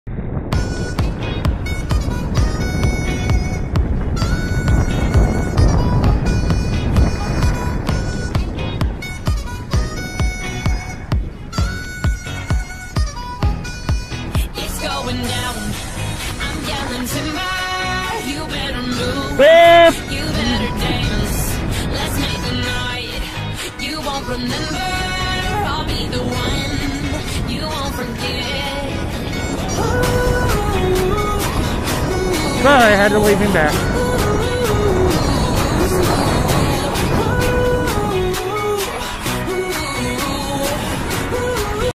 Tim's ski fall
My son TJ and I skiing at Winterplace in WV. That is him in front